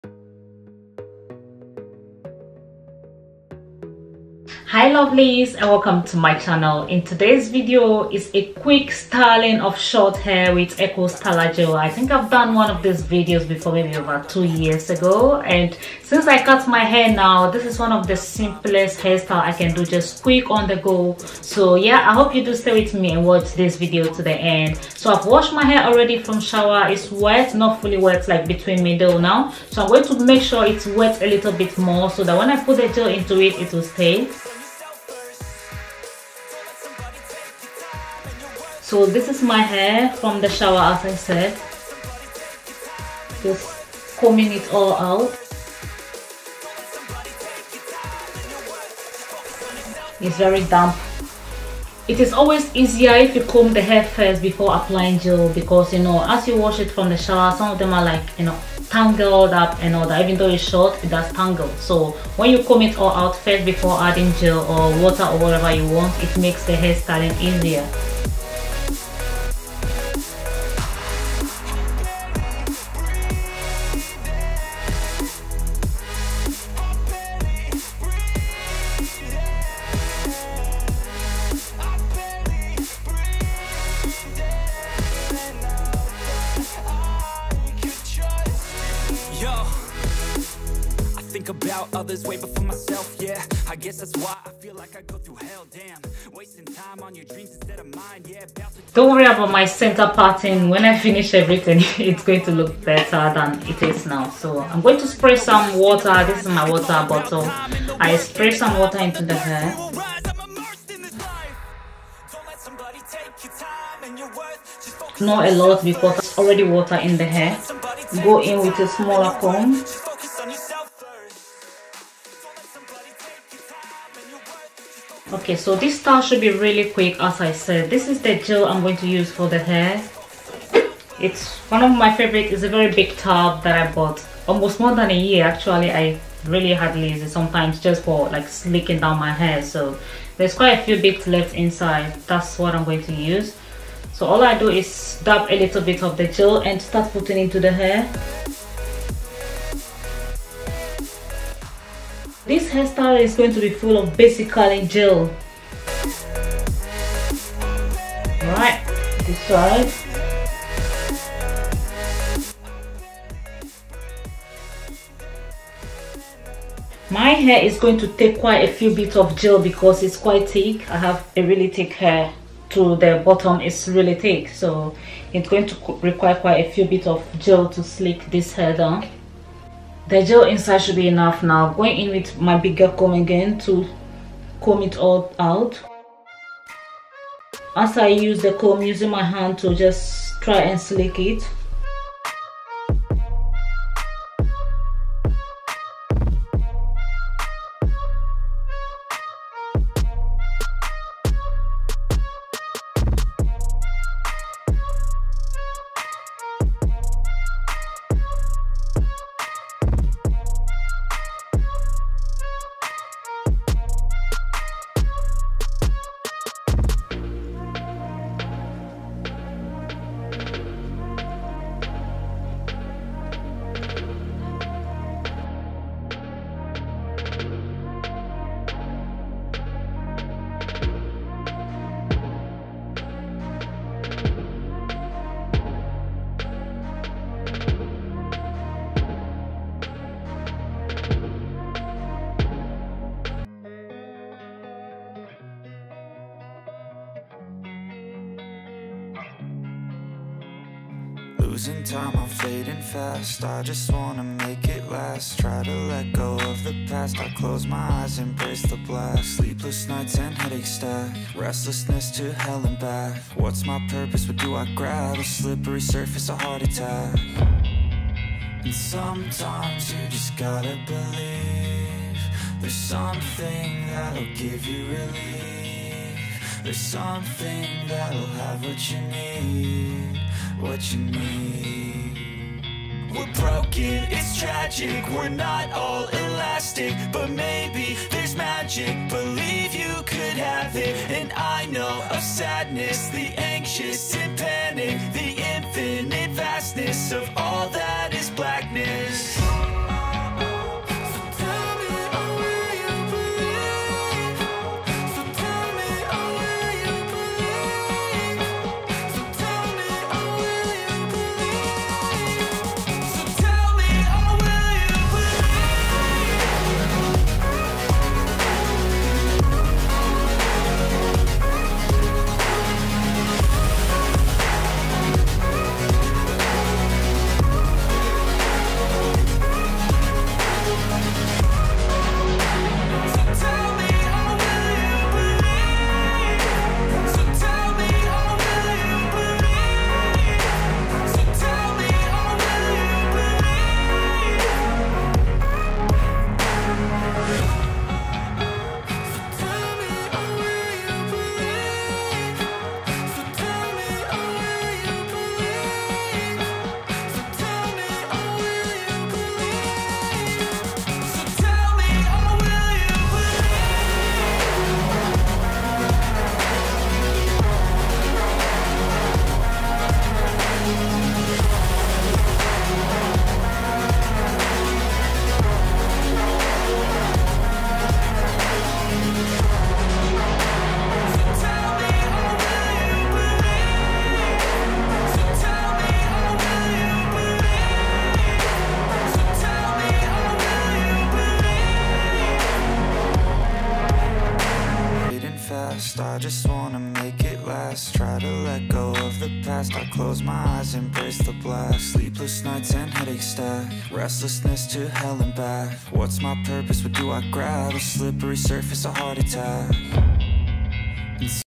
BEGINNERS STYLING SHORT HAIR WITH ECO STYLER GEL
Today’s is beginners styling of short hair using only Eco styler gel. Yes since I cut my hair short I’ve been doing quick simple wash and go styles like this and am loving them. Eco styler gel has been amazing to my short hair.

SIMILAR VIDEOS.

ethnicity : Ghanaian

Location : United Kingdom

editing: iphone 12pro inshort.